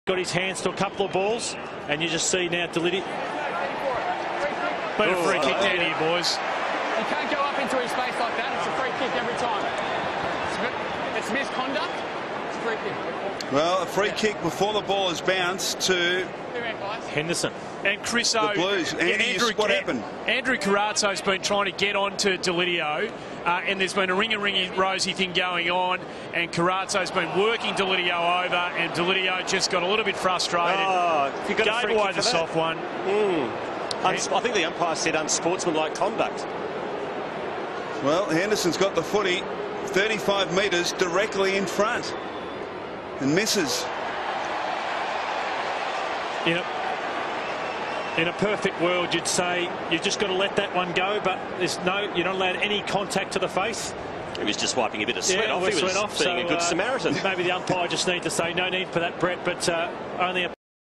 Deledio frustrated by sooky Henderson
Deledio wipes tears from Henderson's face after he had a cry to the umpire